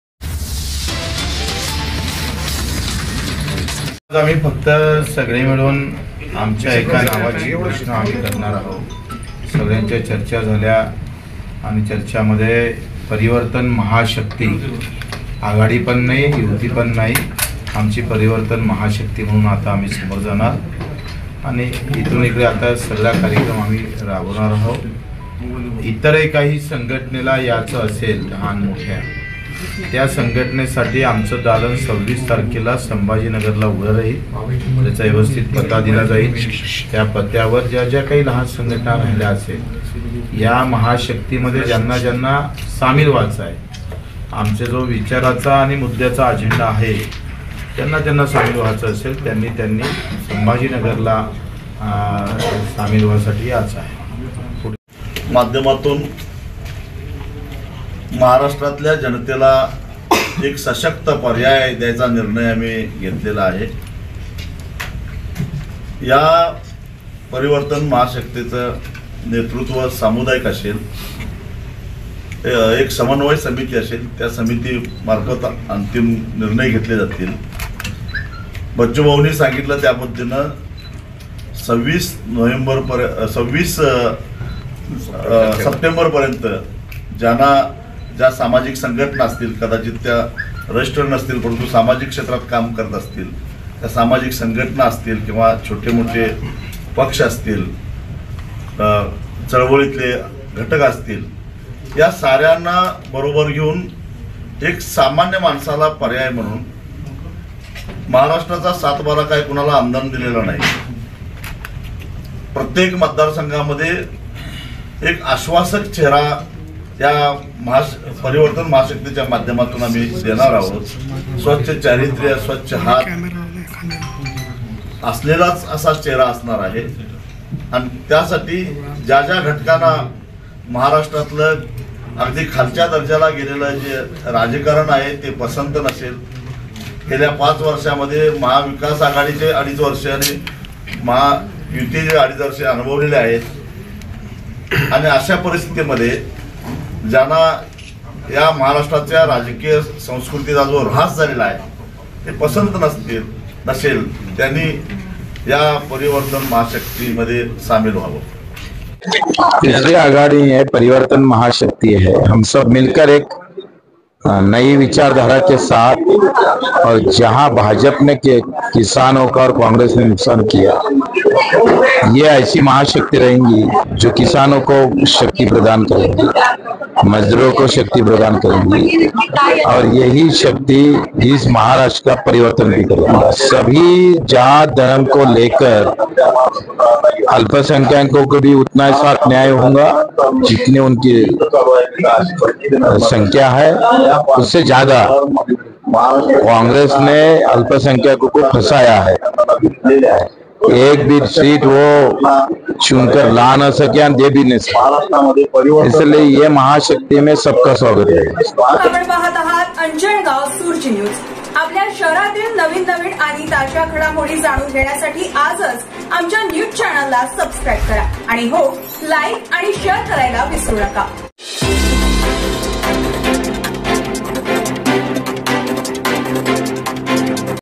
महाराष्ट्रात तिसरी आघाडी 'परिवर्तन महाशक्ति' बच्चु कडू, राजू शेट्टी, संभाजी महाराज...
Anjangaon surji News जावेद शाह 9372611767